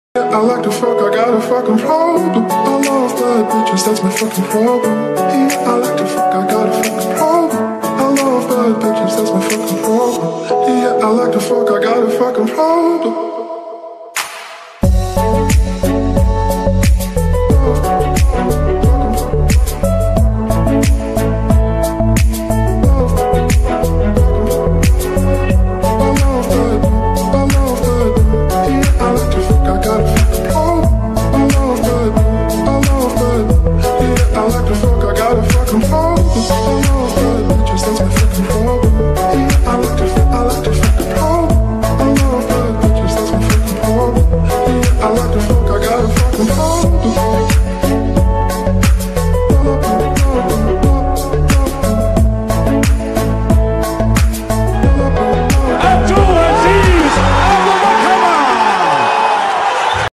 Абдул-Азиз Абдулвахабов vs. Эдуард Вартанян
Титульный поединок
Представляем Вашему вниманию ВИДЕО поединка АСВ 77: Абдул-Азиз Абдулвахабов vs. Эдуард Вартанян, который состоялся 23 декабря 2017 года в Москве.